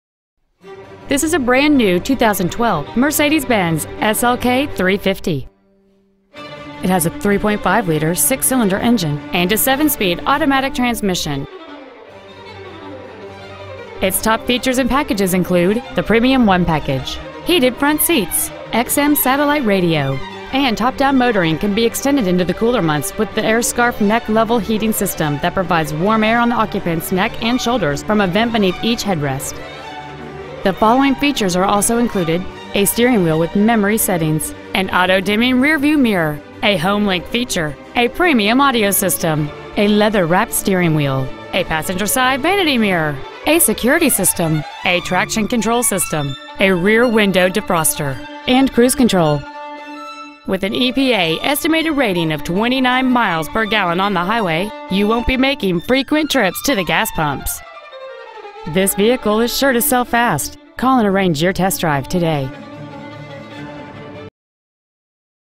2012 Mercedes-Benz SLK350 Roadster Belmont CA 94002
Year : 2012

 Make : Mercedes-Benz

 Model : SLK350

 Engine : 3.5L V-6 cyl

 Trans . : 7-Speed Automatic

 Exterior : Steel Gray Metallic

 Miles : 8

 Interior : Black

 Autobahn Motors

 700 Island Parkway

 2012 Mercedes-Benz SLK350 Roadster Belmont CA